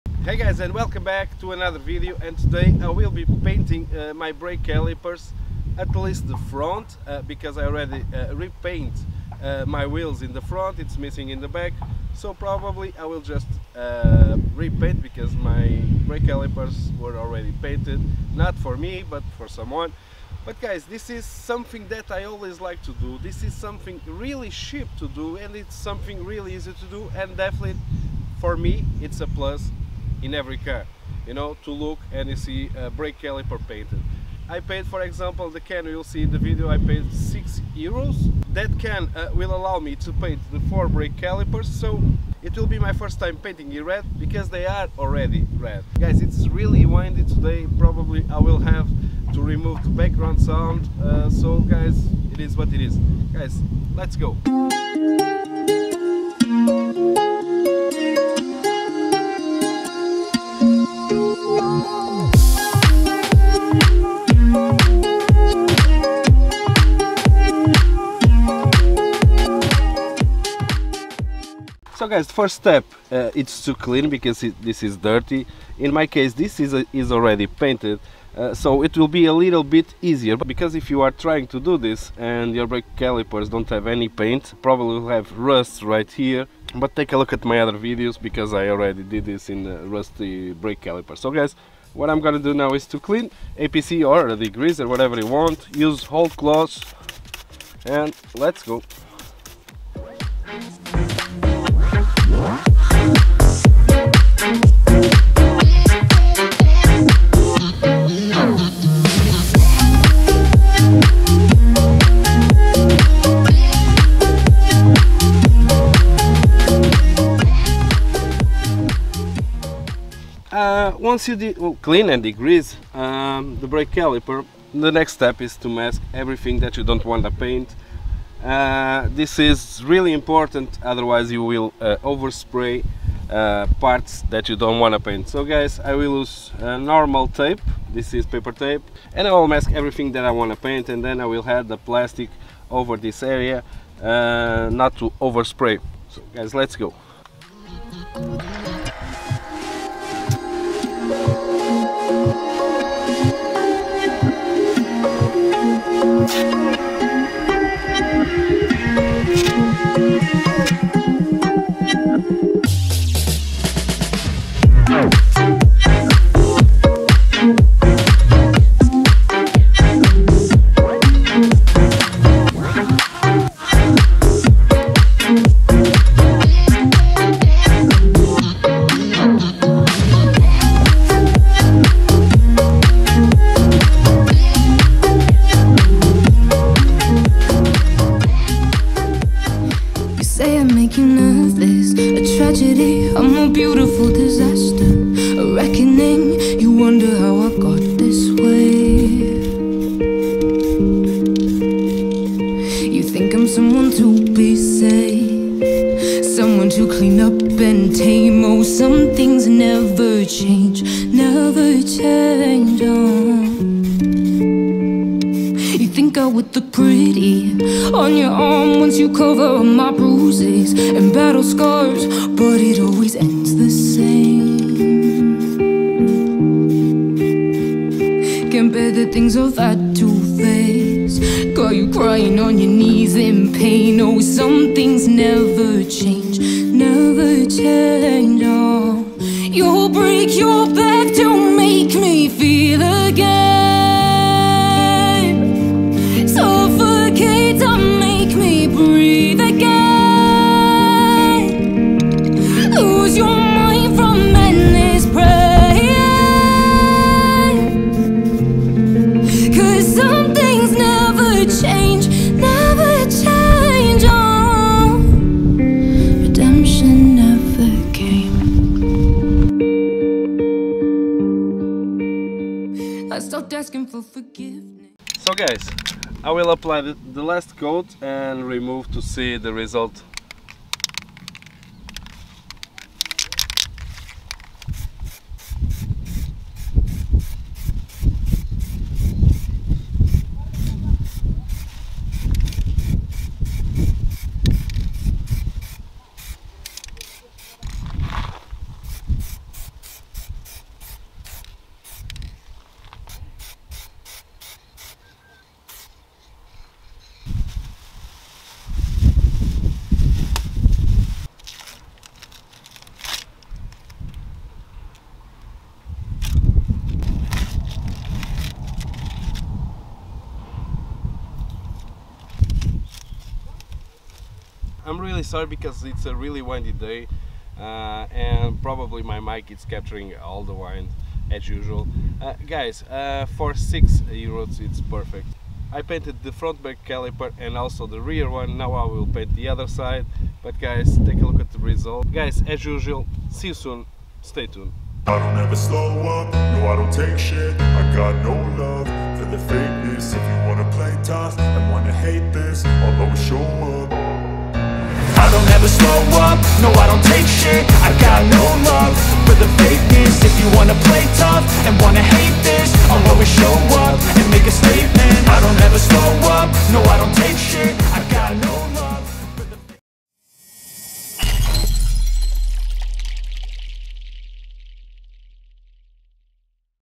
✅ Audi A3 8L Cheap Brake Caliper Paint
Hey guys,

I'm still working on the Audi A3.

At this moment I'm waiting for important parts to arrive for the engine, meanwhile I decided to give a refresh on the brake calipers.

My brake calipers were already painted, but the paint was old and ugly.

I bought a can of k2 paint for calipers and did a quick repaint.

This cost me just over €6, which is about €1.5 for each caliper.